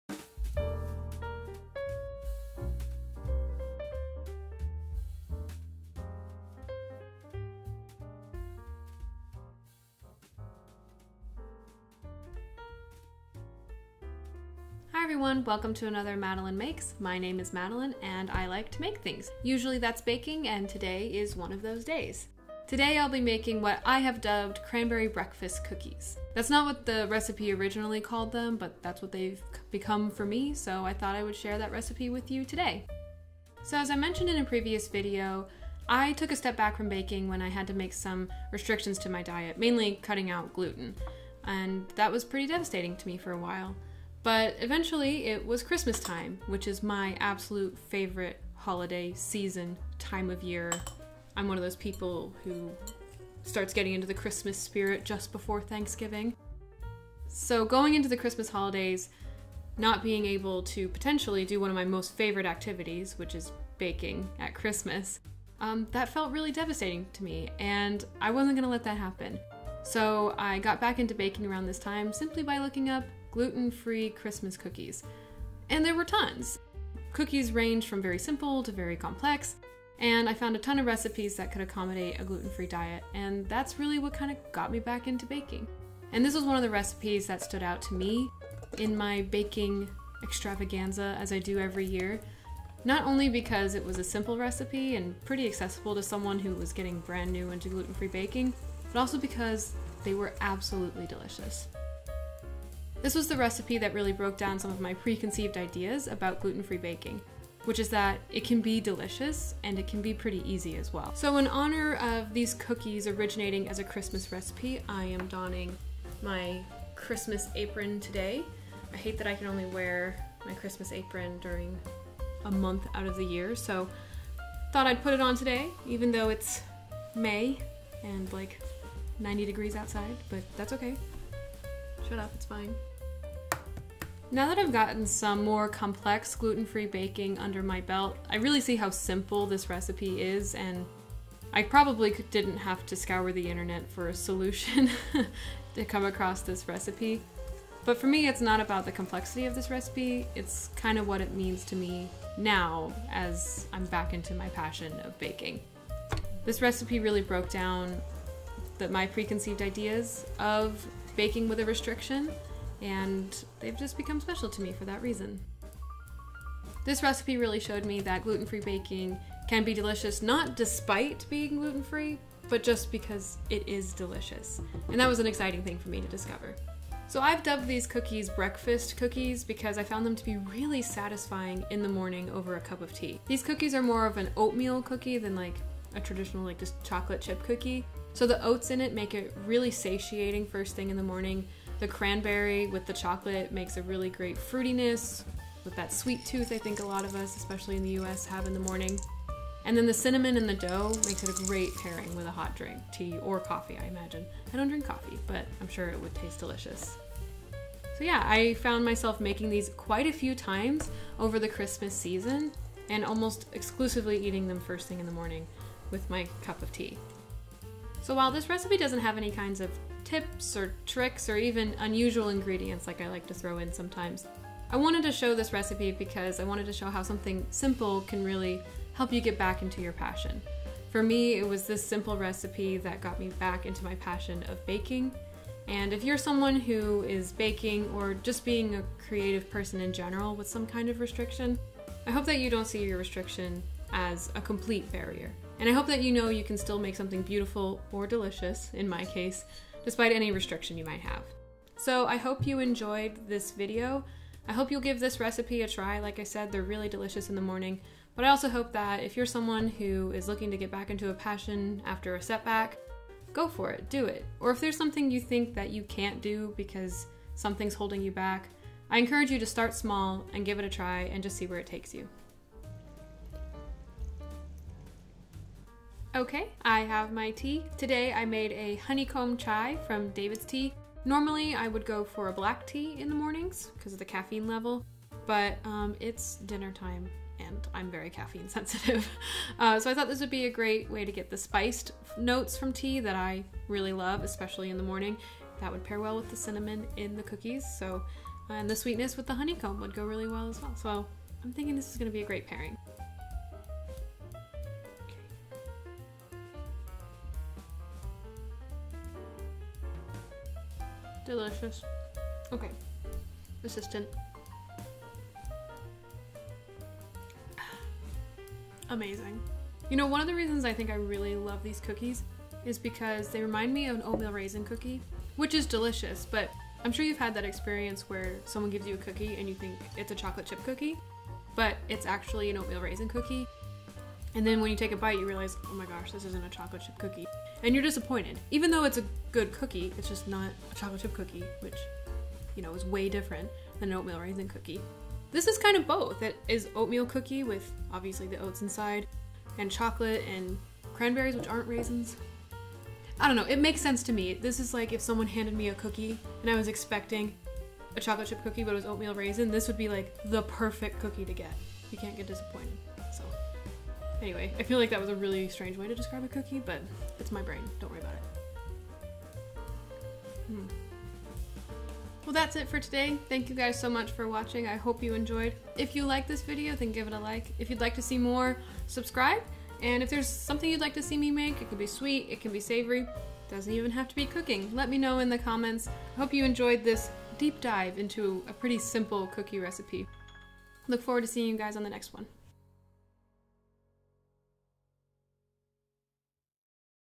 Madeline Makes Oatmeal Cranberry Chocolate Chip Breakfast Cookies
Welcome to another episode of Madeline Makes! Today, I made a simple but impactful recipe: Oatmeal cranberry breakfast cookies! This recipe is simple and delicious but, through my own journey navigating a gluten restriction, it has become a special recipe to me. I hope you give it a try and enjoy!